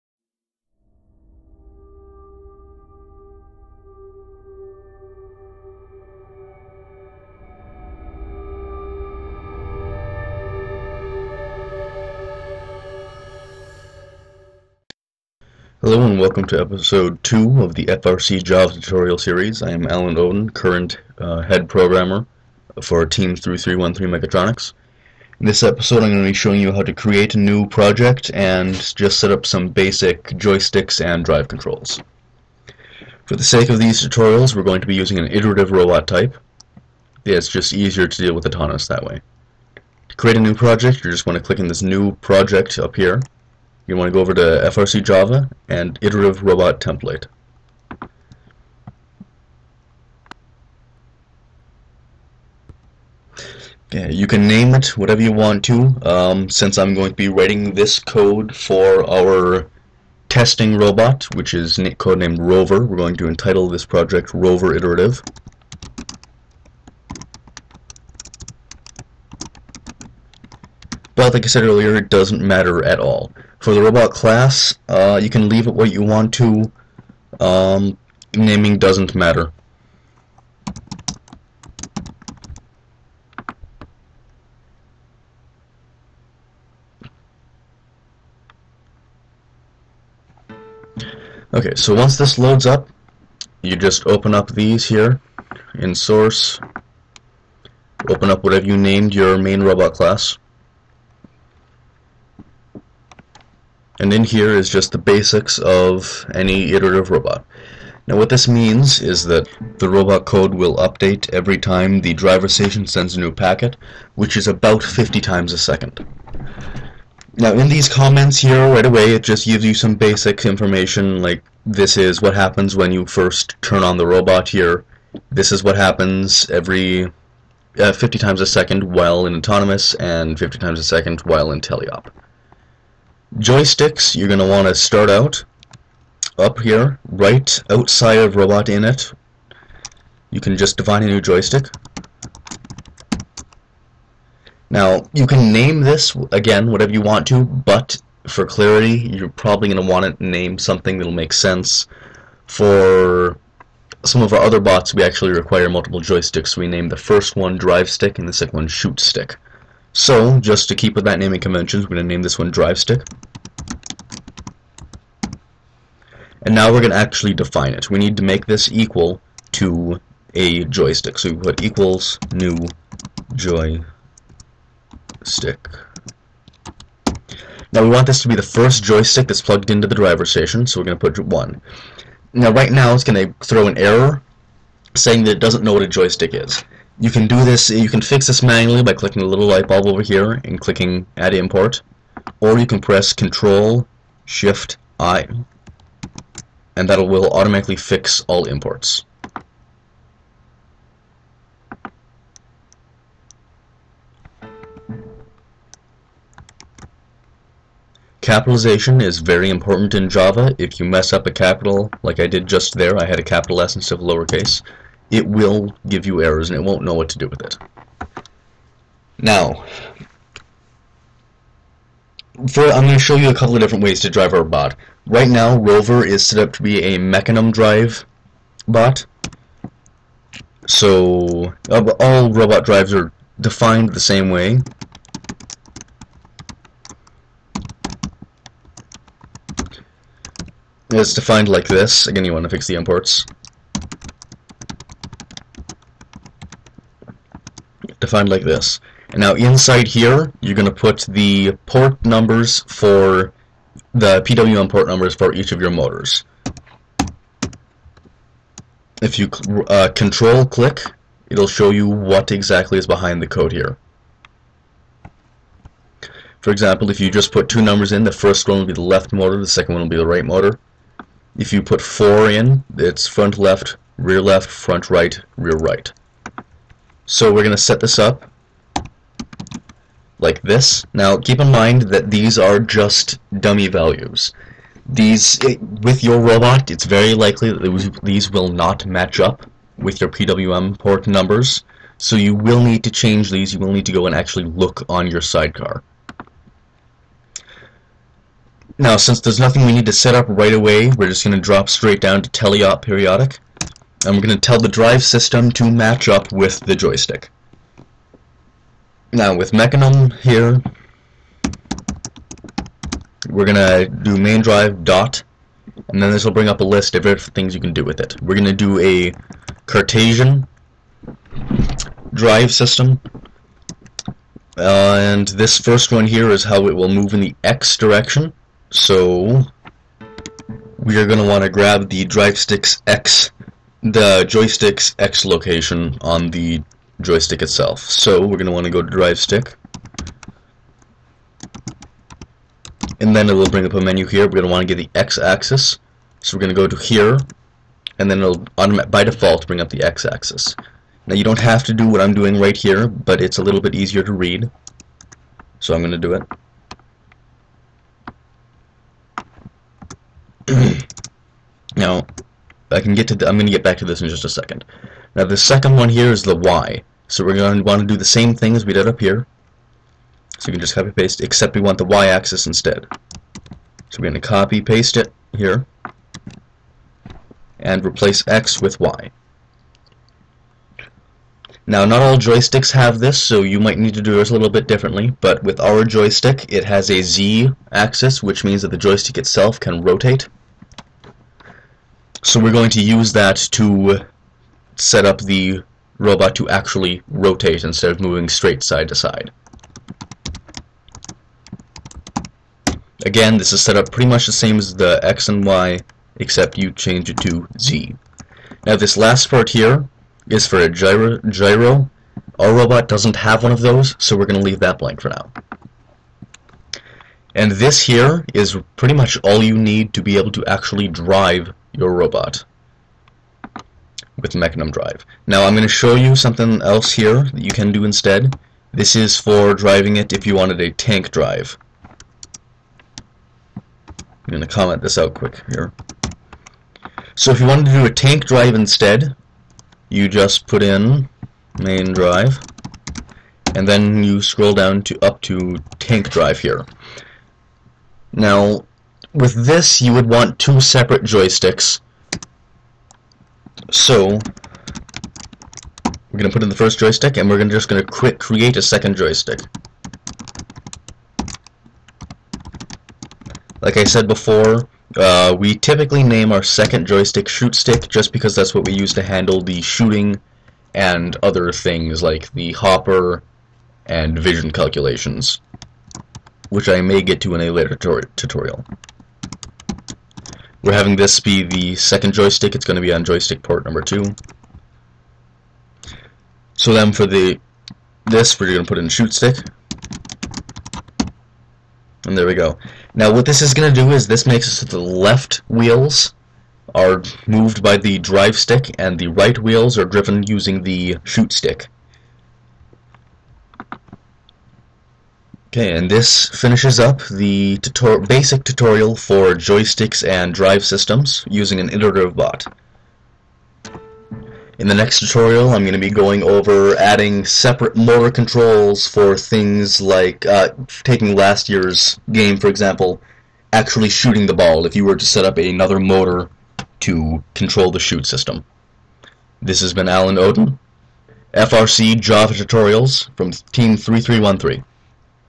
FRC Java 2 - Joystick and Drive Systems
In this episode I will be covering creating an iterative bot and making it drive.

Check out the team blog: team3313mechatronics.blogspot.com